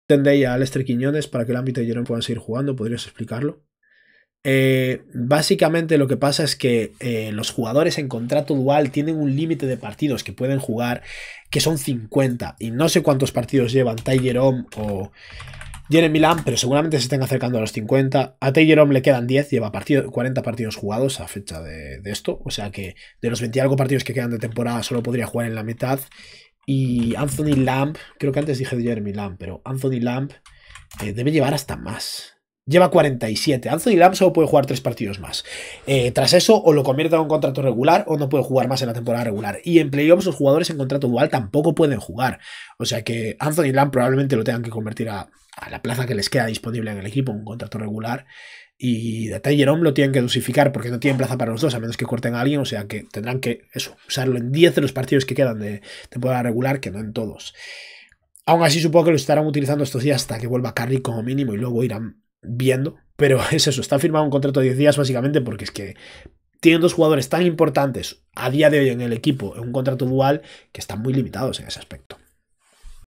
¿Por qué no juegan en todos los partidos Ty Jerome y Anthony Lamb? | BASKET 365
Clip en el que explicaré por qué los Golden State Warriors tienen que dosificar las apariciones de sus 2 jugadores en contrato dual en Ty Jerome y Anthony Lamb.